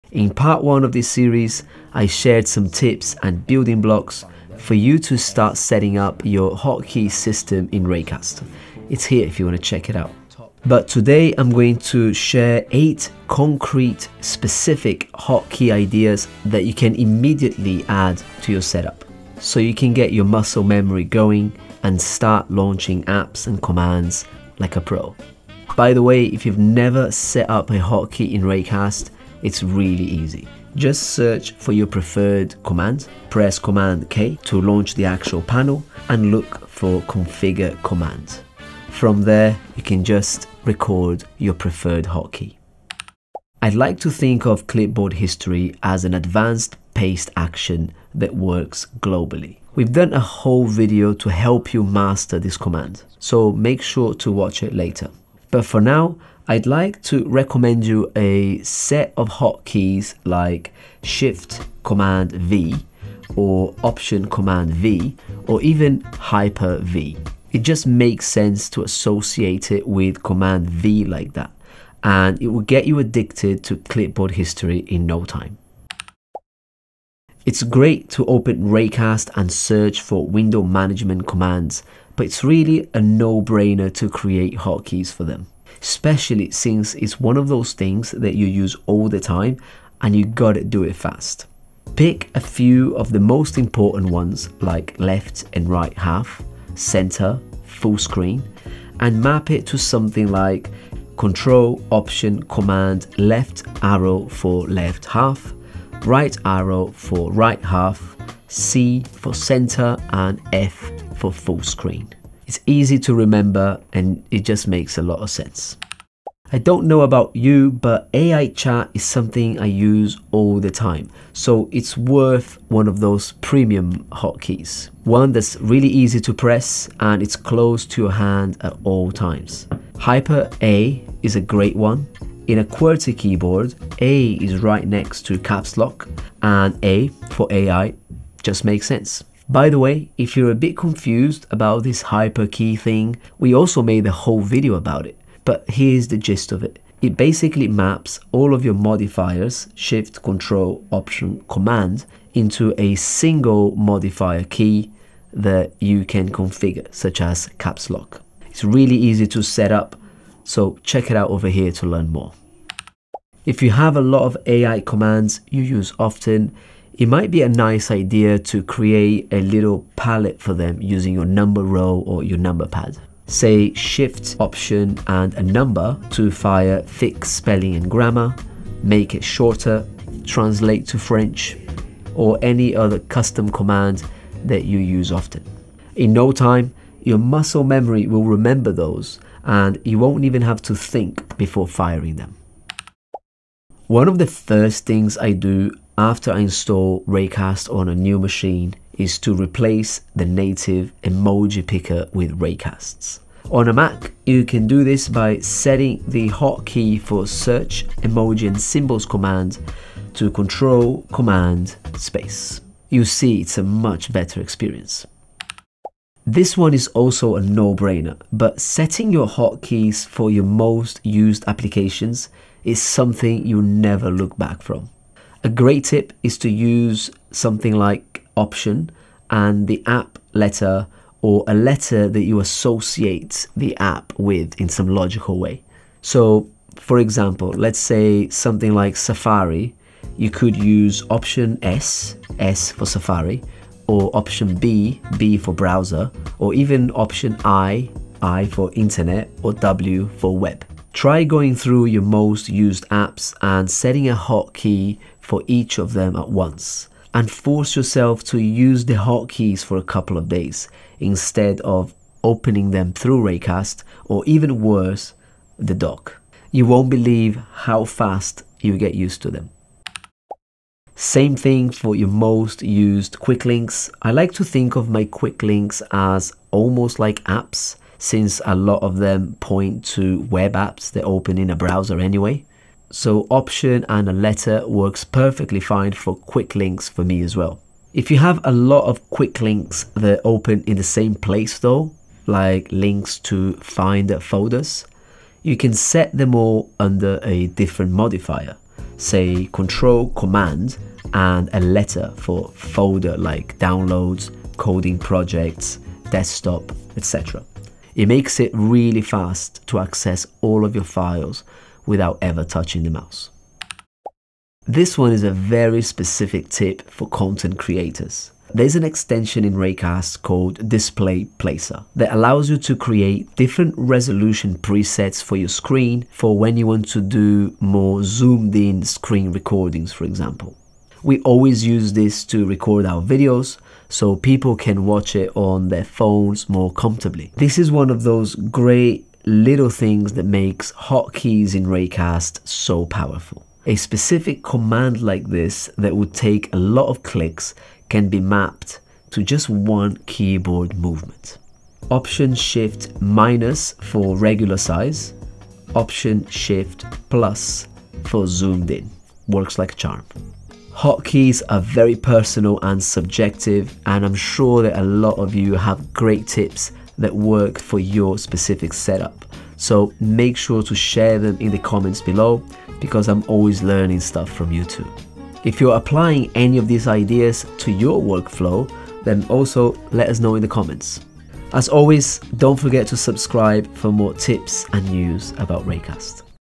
What Hotkey Should I Use? | Raycast Hotkeys Guide Part 2
After setting up your hotkey mental map like we helped you with in the first part, here's 8 concrete hotkey ideas you can immediately add to your workflow.